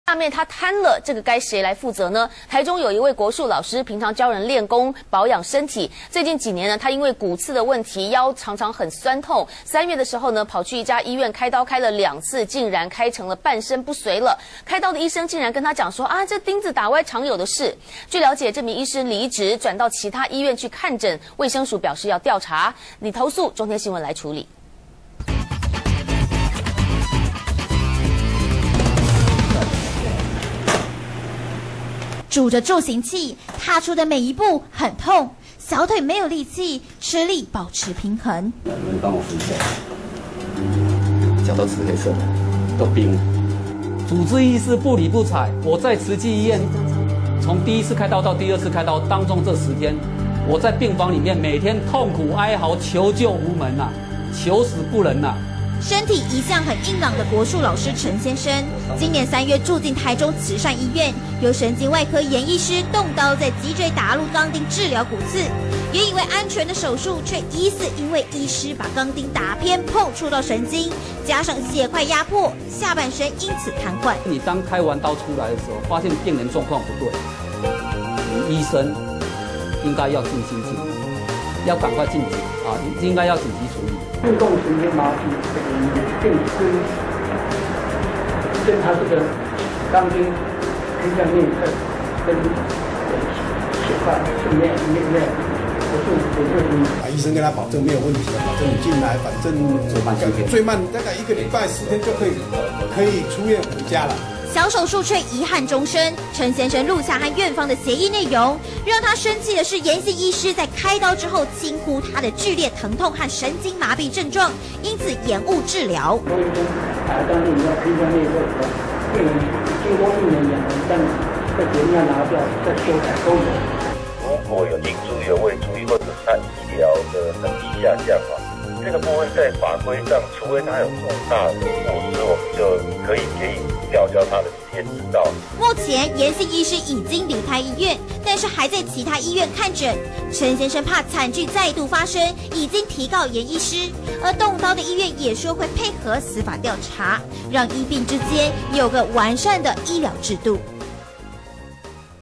骨刺開刀半癱 醫竟稱「釘子打歪常發生」
中天新聞》台中有一位國術老師，平常教人練氣功保養身體，近幾年因為骨刺問題腰部常常酸痛。今年三月到一家慈善醫院去開刀治療，前後開兩次刀，竟然變成半身不遂。而開刀的醫師竟說，釘子打歪常有的事。據了解這名醫師離職，轉到其它醫院看診，衛生署表示要調查！